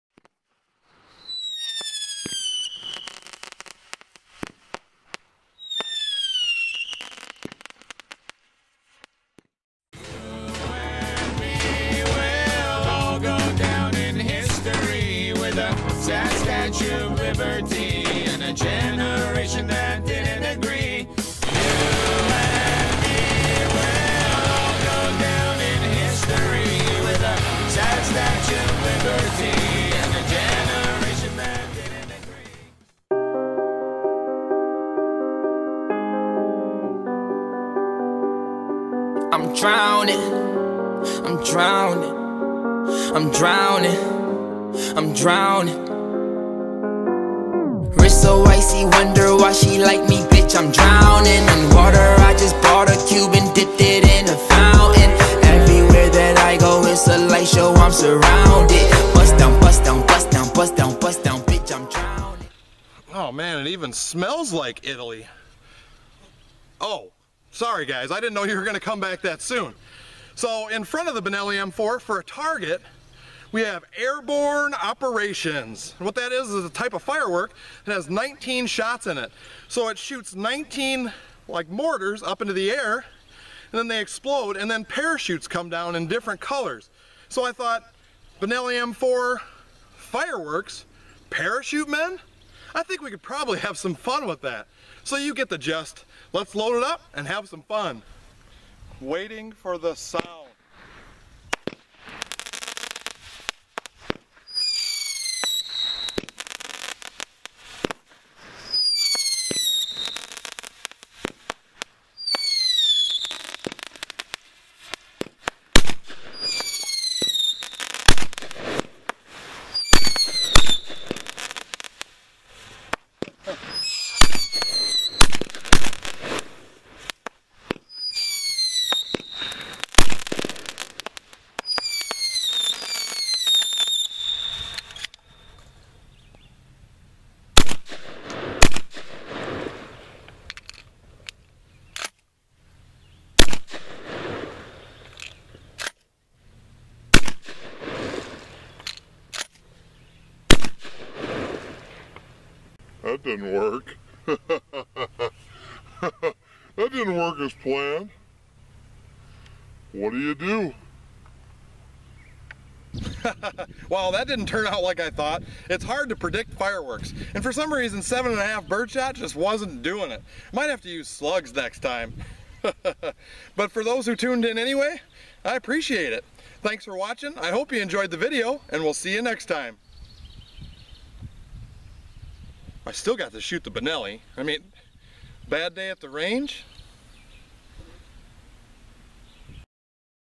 Shooting Fireworks With A Shotgun
Trying to shoot fireworks out of the air with the Benelli M4 shotgun.